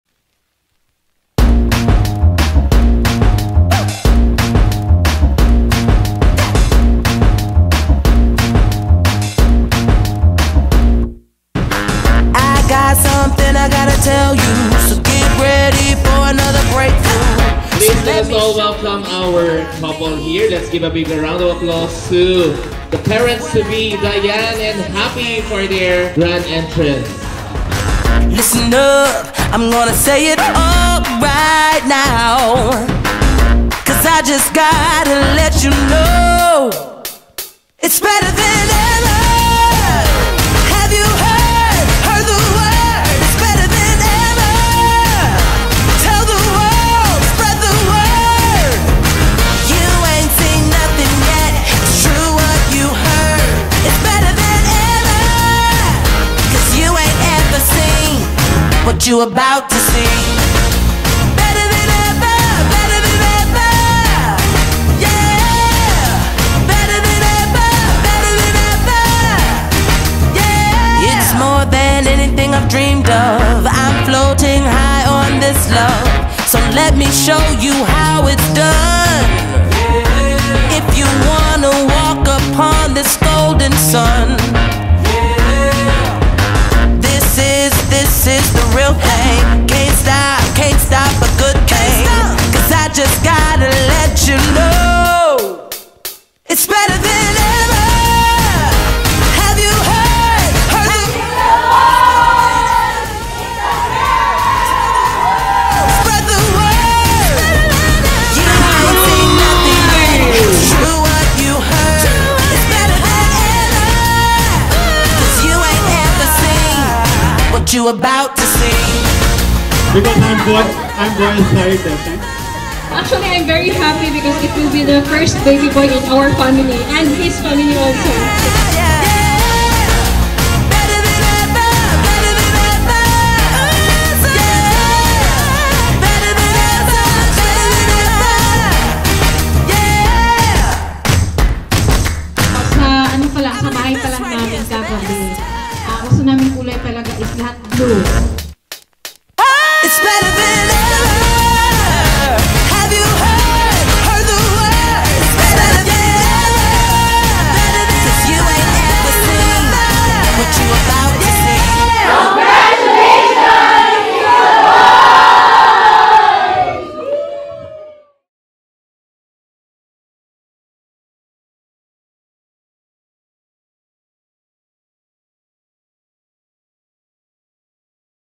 Our Baby Gender Reveal 💙🩷👶🏻 | Filipino🇵🇭 - Punjabi🇮🇳 Parents | Singh or Kaur?
Our Baby Gender Reveal
Singh or Kaur?
Can you guess it?